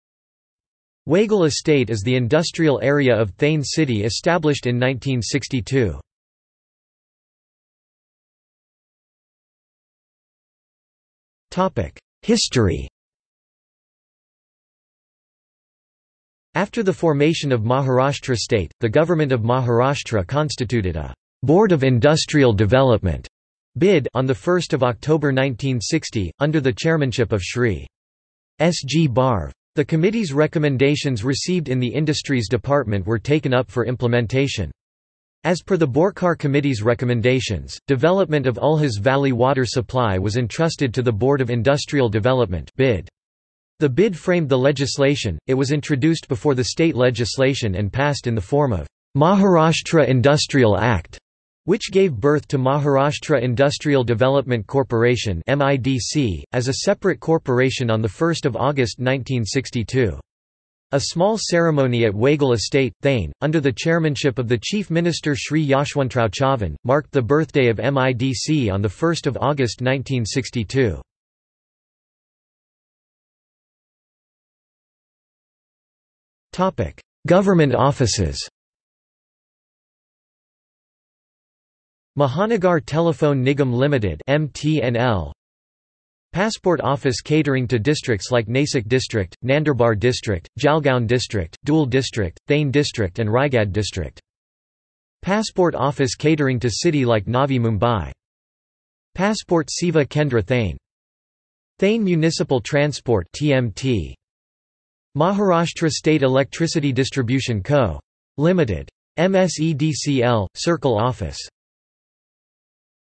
Wagle Estate | Wikipedia audio article
This is an audio version of the Wikipedia Article:
Wagle Estate

00:00:07 undefined
00:01:12 undefined

- Socrates

SUMMARY
Wagle Estate is the industrial area of Thane city established in 1962.